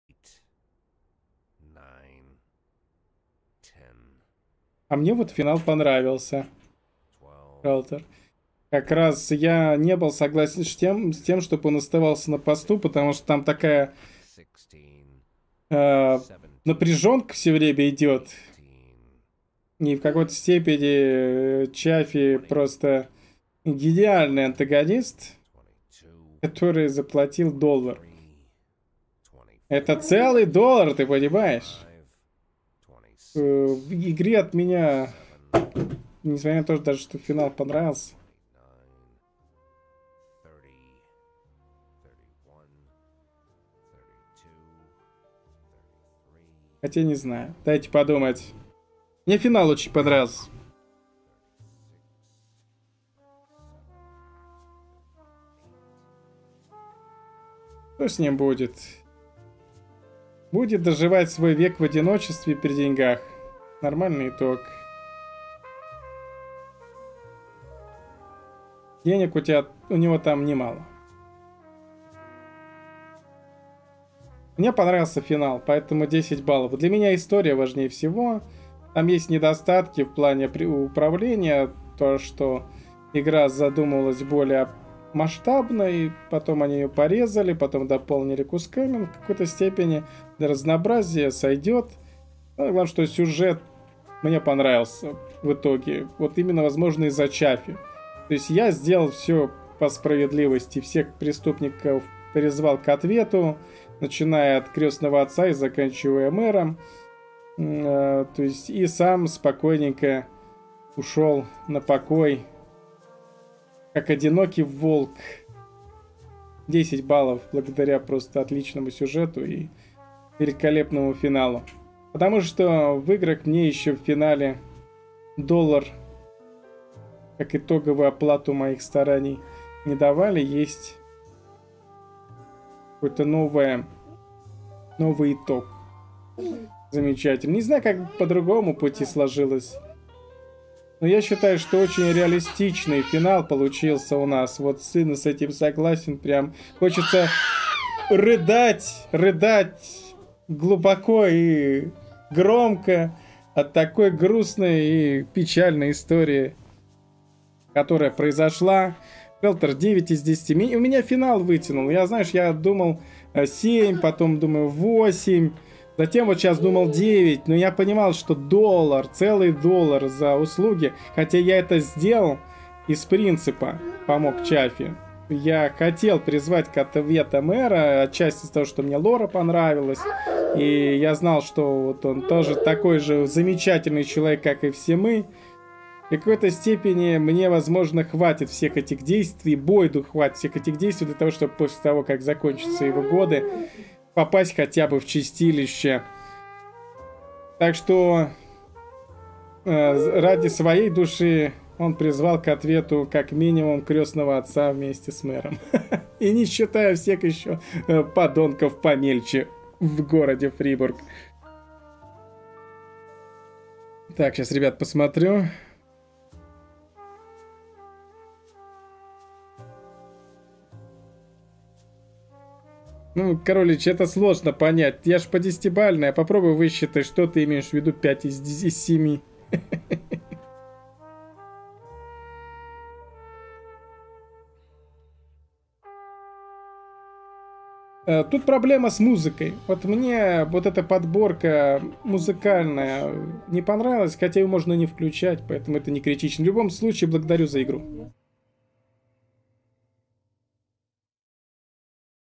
Мнение о This Is the Police.
Берём шефство над полицией, итог впечатлений.